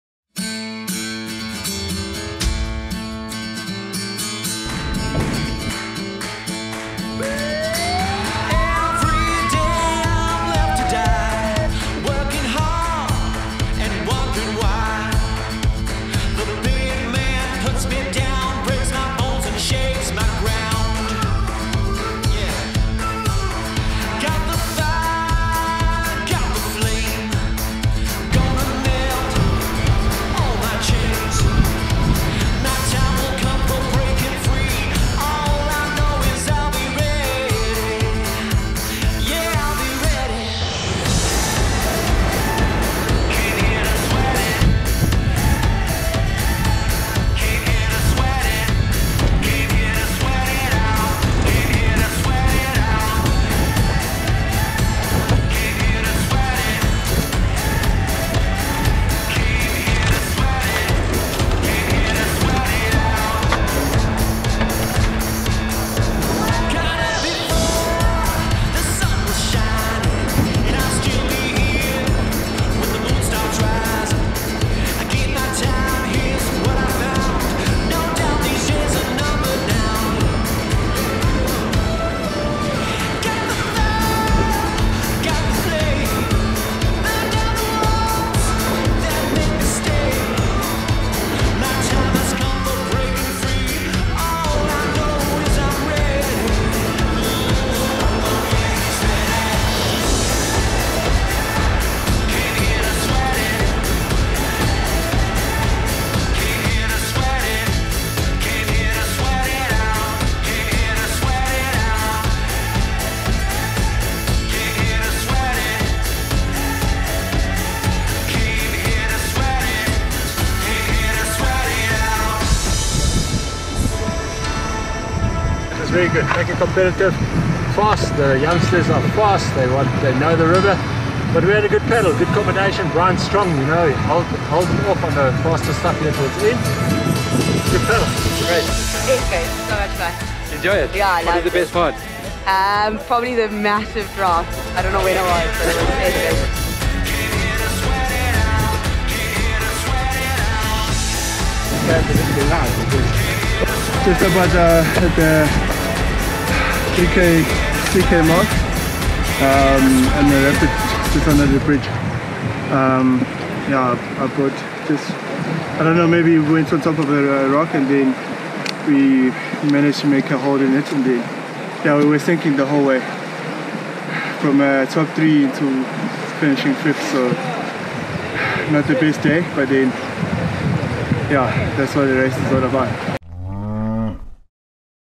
VKB Liebenbergsvlei Canoe Marathon 2019
A fun day out on the Liebenbergsvlei River as 350 paddlers enjoyed fresh clean water out of the Katse Dam.